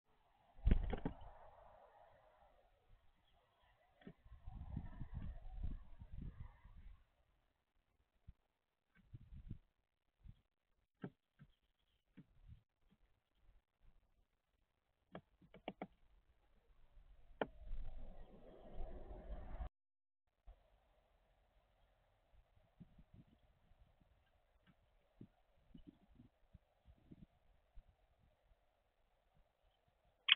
New great spotted woodpecker visited the bird feeder camera.
New great spotted woodpecker visited the bird feeder camera looks like mr wodix is successful this year welcome to the world little one.